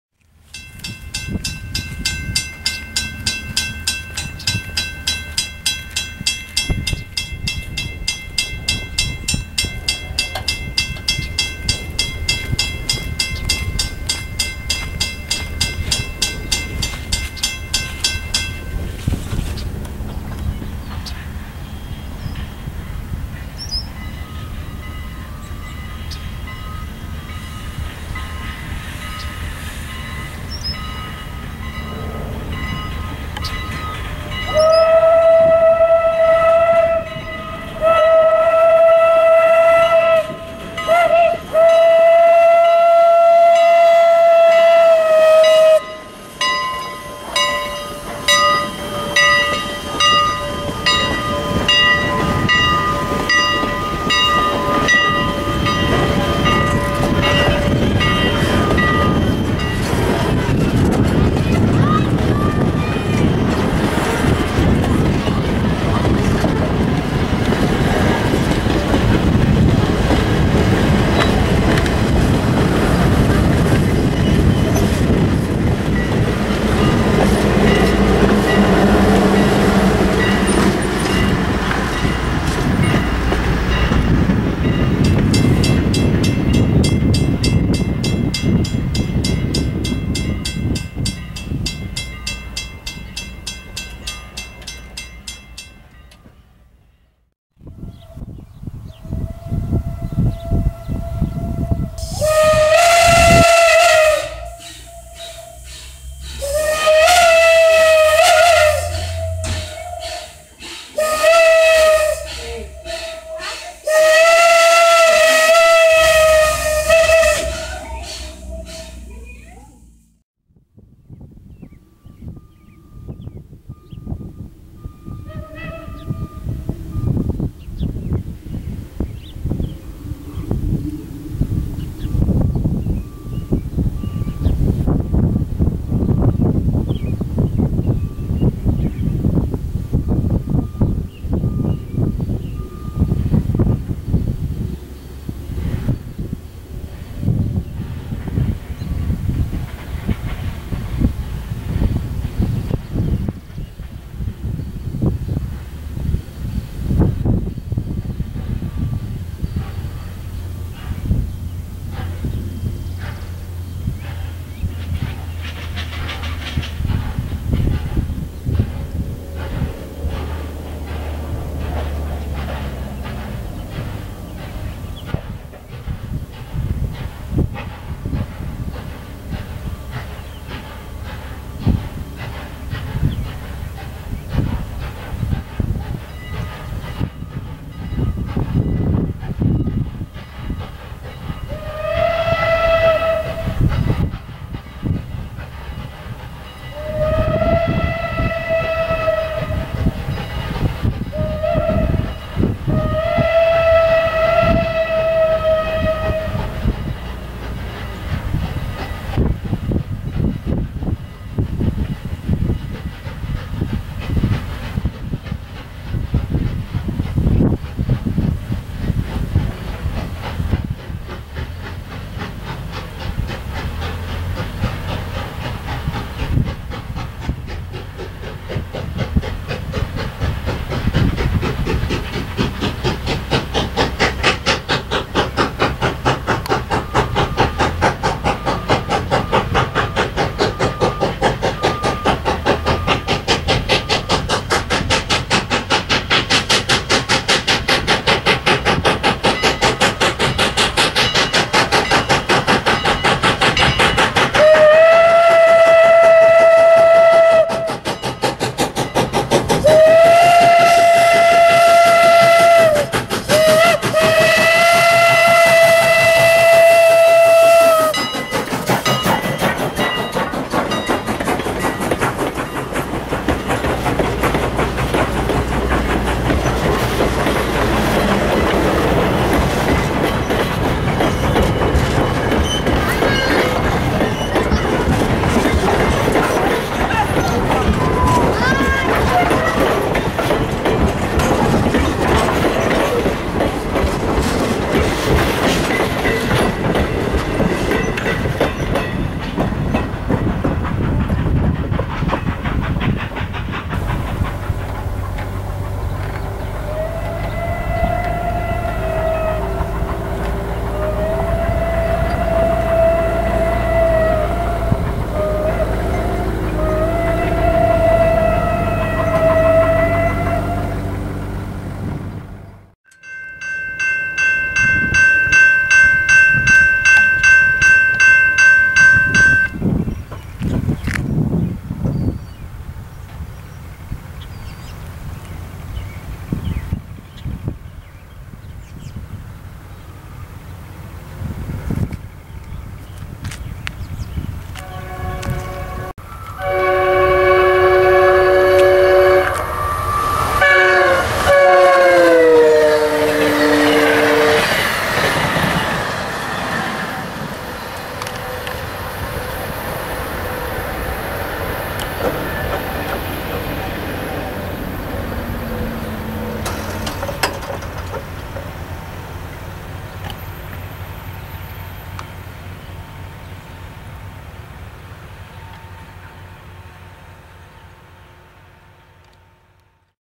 Strasburg Rail Road - #89 & The 9 Car Train
Recorded May, 30th 2013. Additional footage is an Amtrak train.